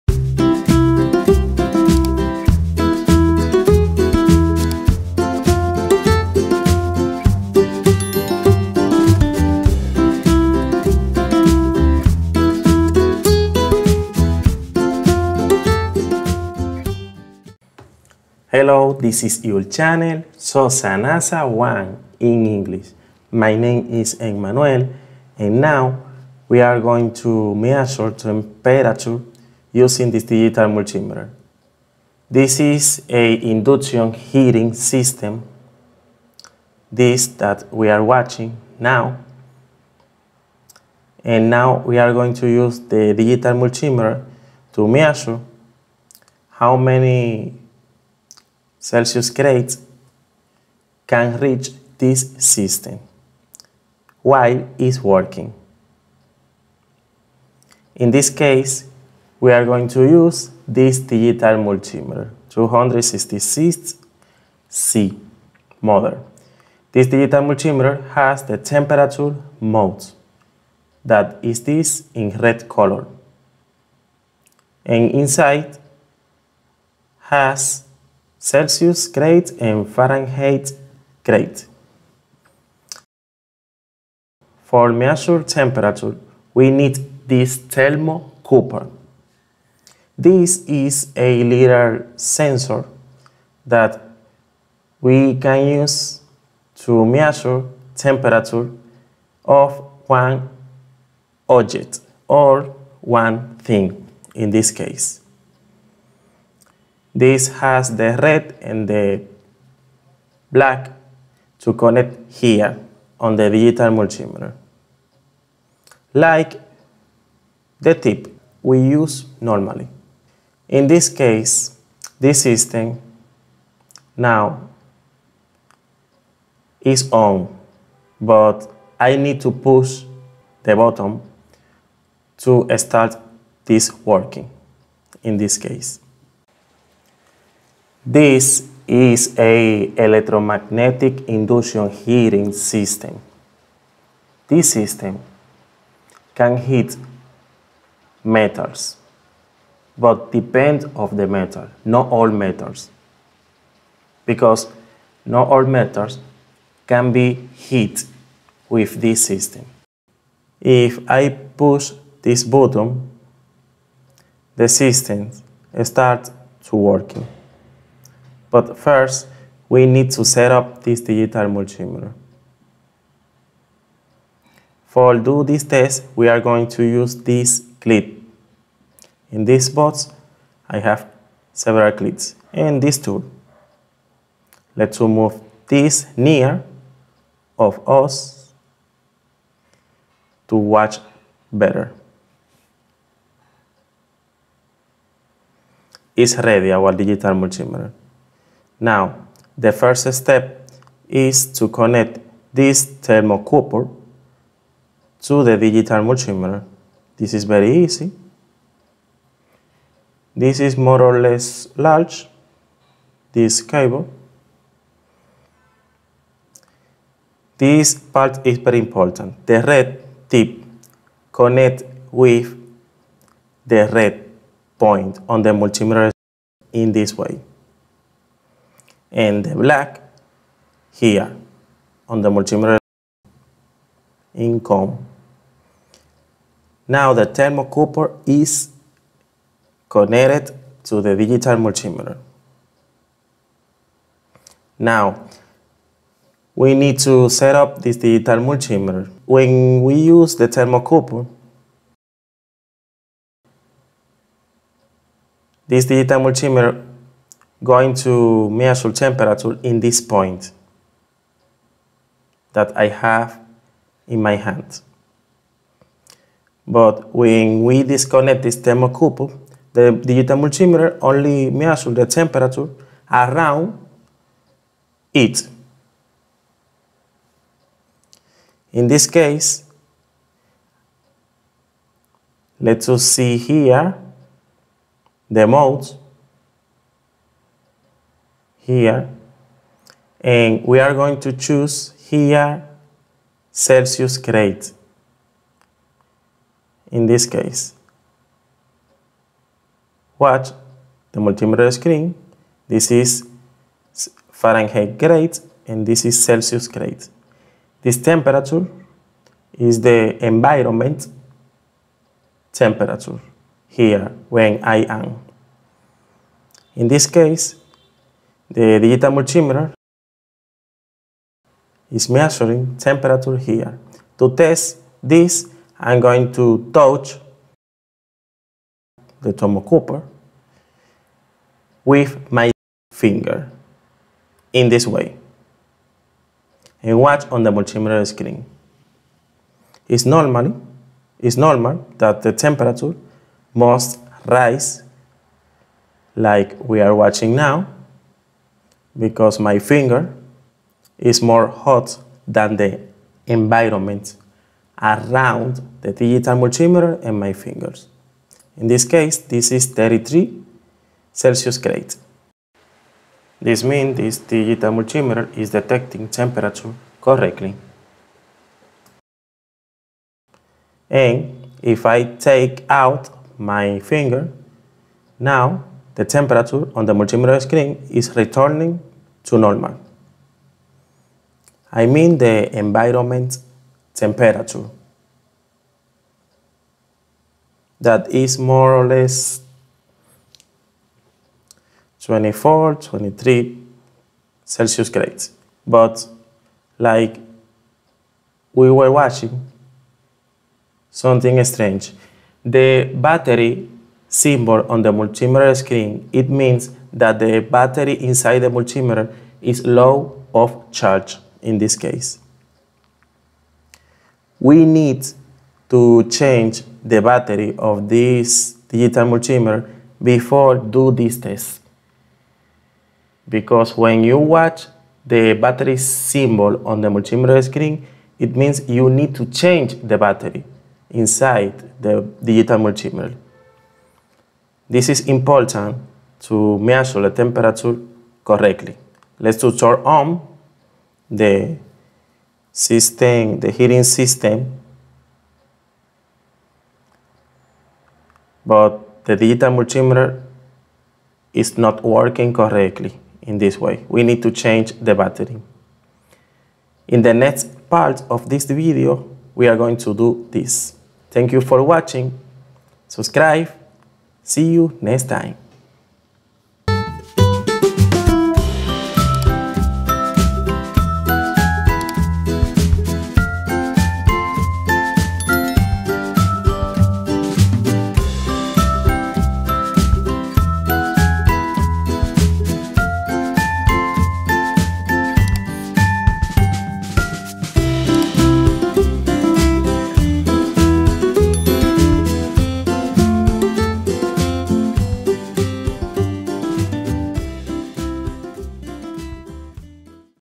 how to test the highest temperature of induction heating system with digital multimeter (part 1)
now i'm going to measure temperature with this digital multimeter, i'm going to test the highest temperature inside a induction heating system (part 1).